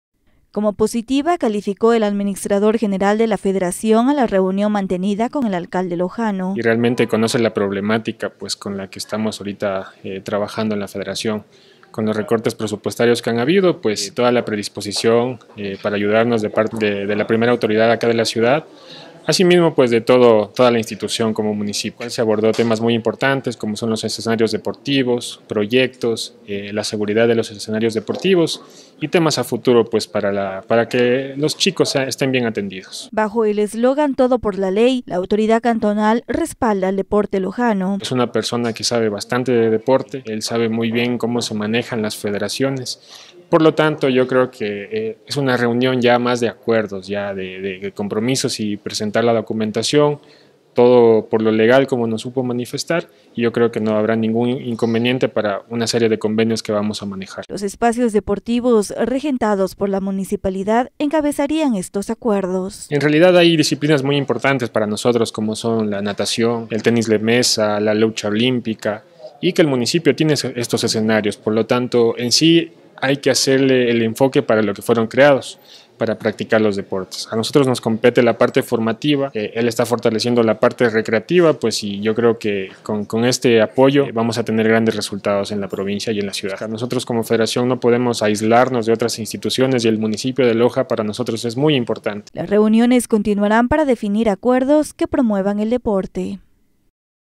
Escenarios deportivos, fueron parte de los temas tratados por los dirigentes de la Federació
Escenarios deportivos y proyectos a futuro, fueron parte de los temas tratados por los dirigentes de la Federación Deportiva Loja-Ecuador y el alcalde Jorge Bailón - Oficial